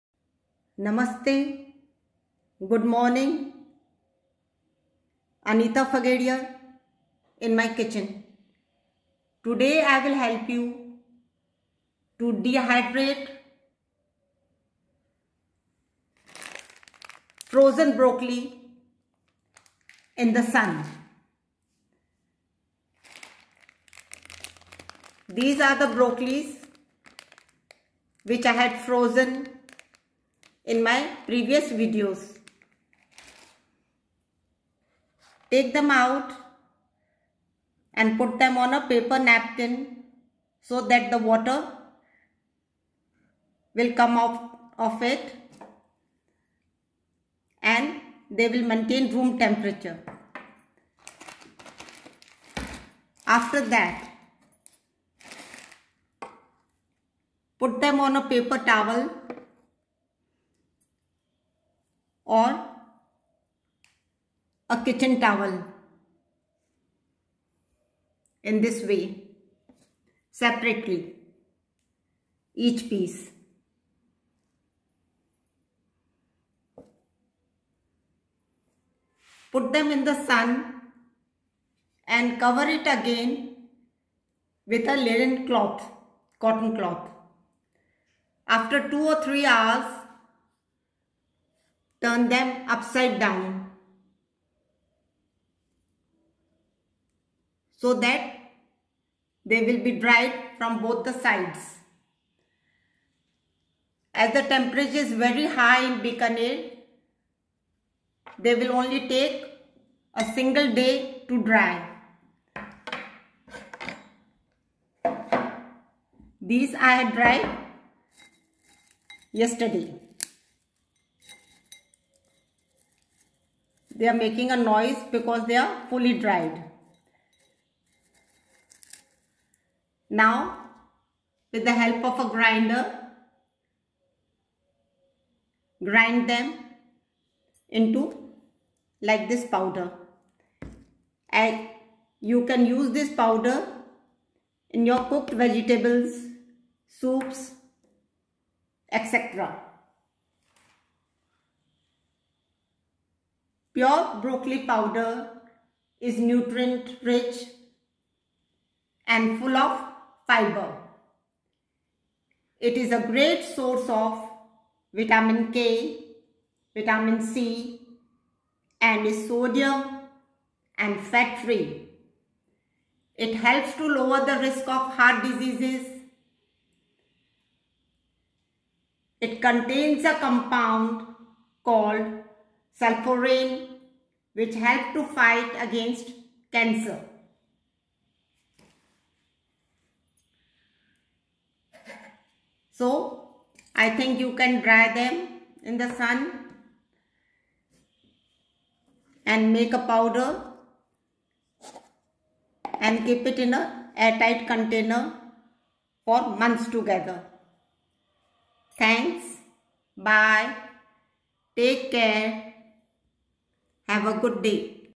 Dehydrating Frozen Broccoli in the Sun by Anita Fageria
Rich in nutrients ,vitamins and fiber.